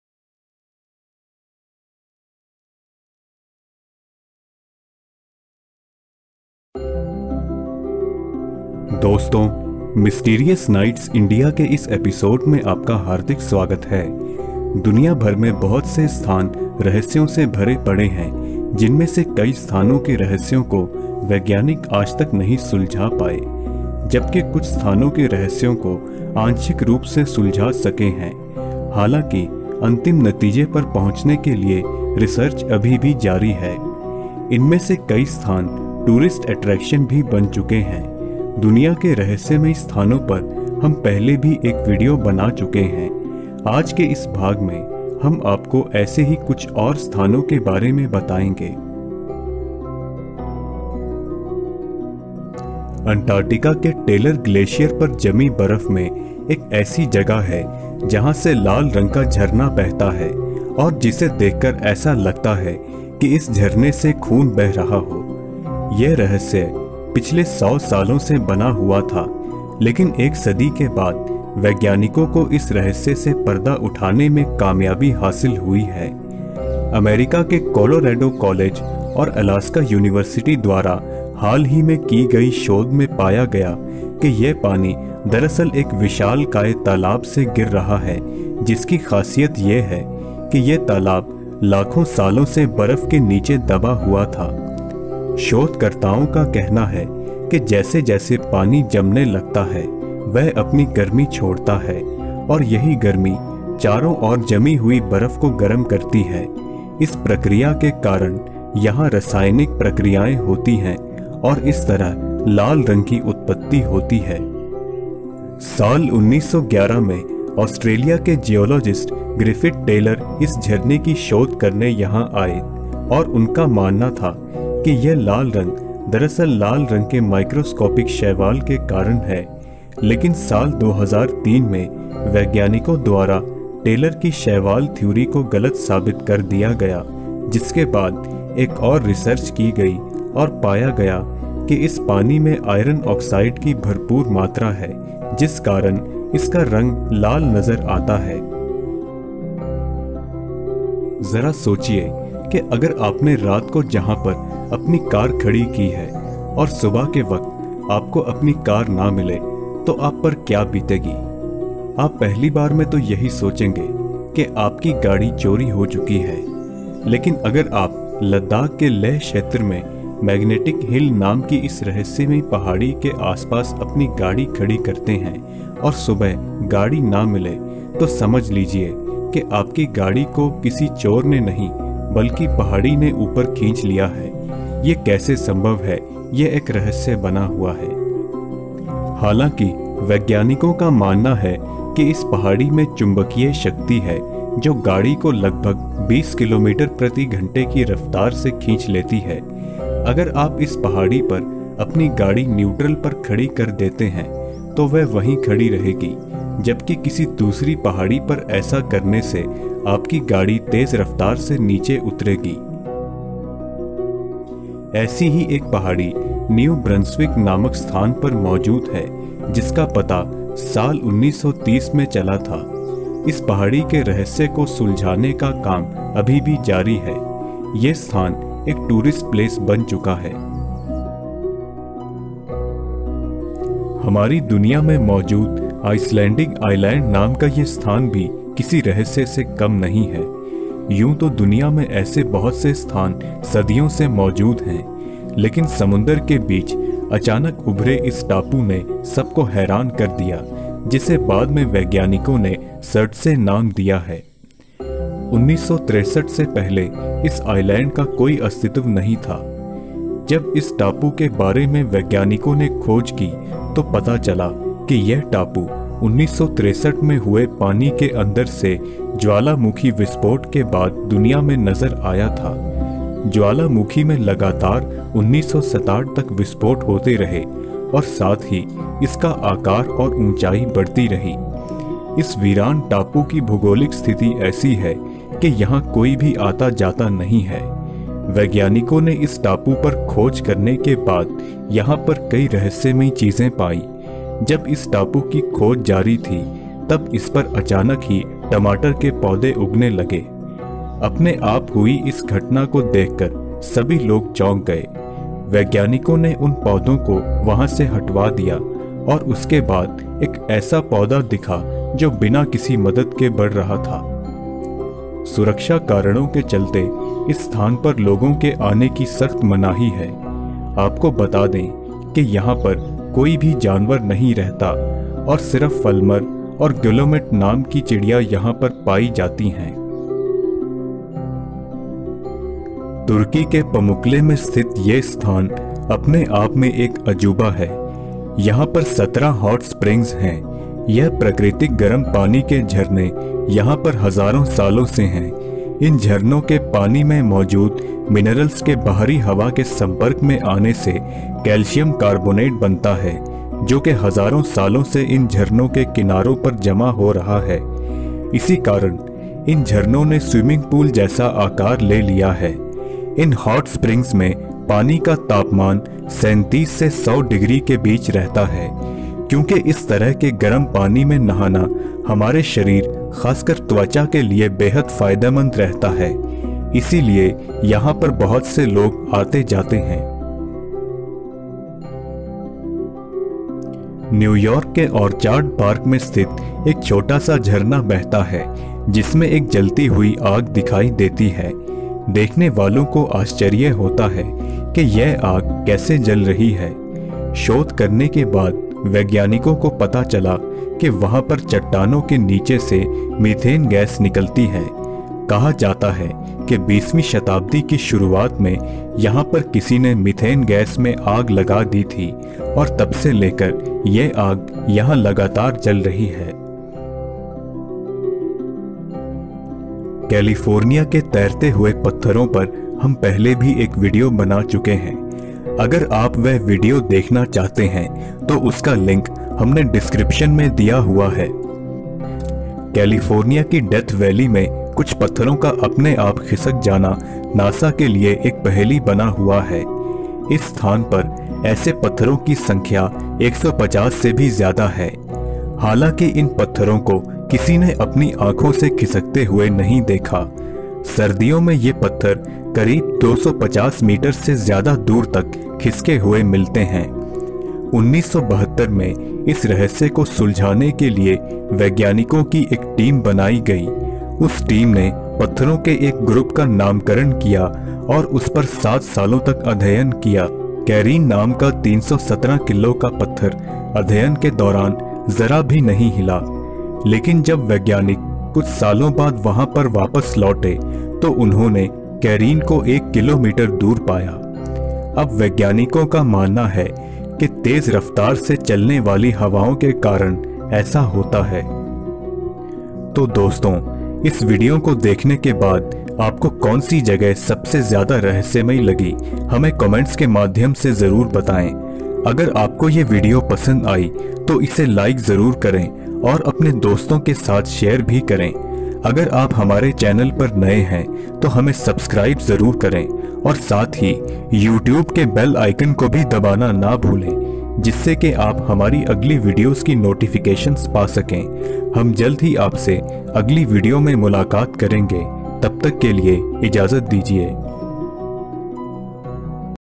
दुनिया के सबसे रहस्यमय स्थान || Part 2 || Mysterious Nights India || Episode – 29#
Hello friends, in todays video, we will be talking about the world's top mysterious places. we have made a video similar to this. here we will talk about some more mysterious places namely -

6. Blood falls of Antartica
5. Magnetic hill, Ladakh
4. Surtsey Island, Iceland
3. Thermal pools, Pamukkale, Turkey
2. Eternal flame falls, Orchard Garden, New York, and
1. Moving stones, Death valley, California

To know more about these places, please watch this video till end.